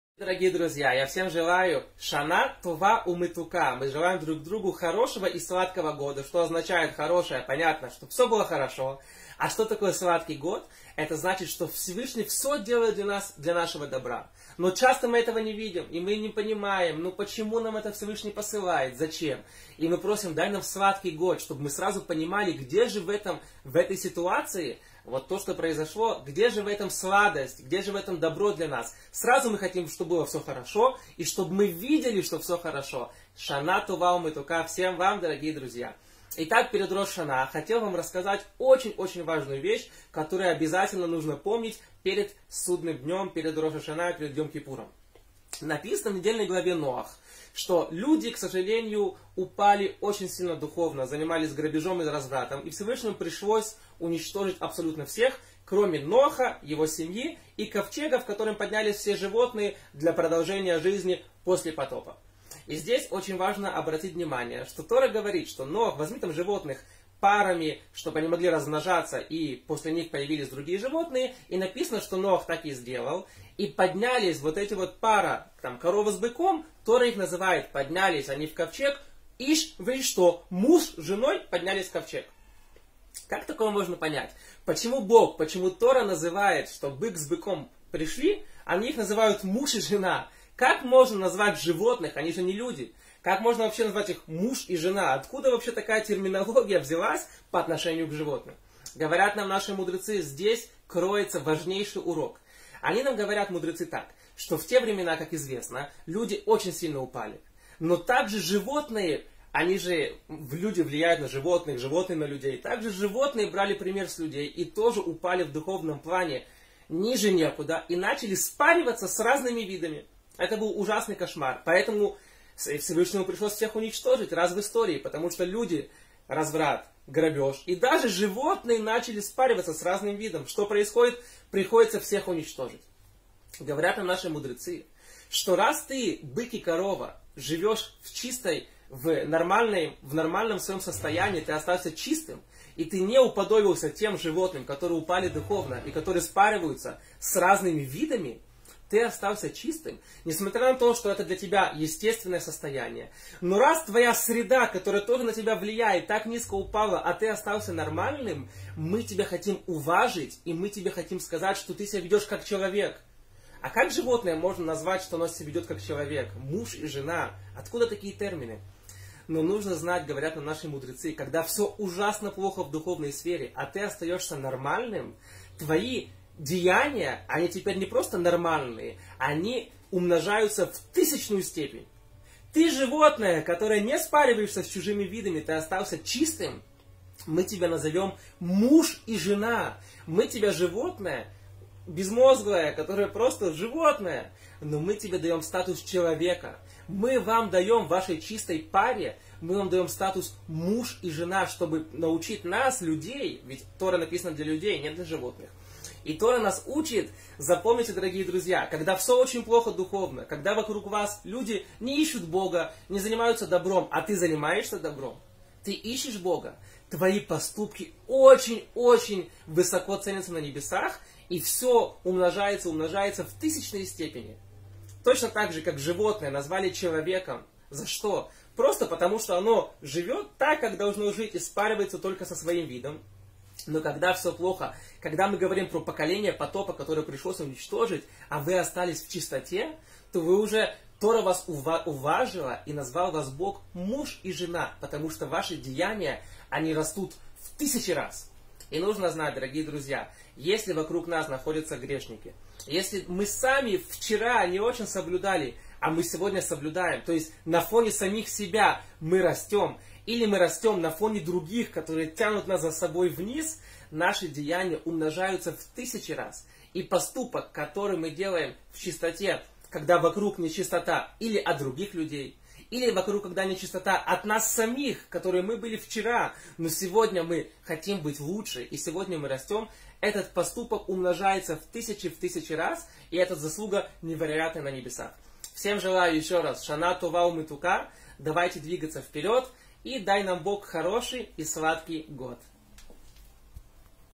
Готовимся к Рош а-Шана. Продолжение | Урок от директора портала VAIKRA р. Яакова Шатагина
За счет чего хороший поступок превращается в СУПЕР-хороший? Смотрим!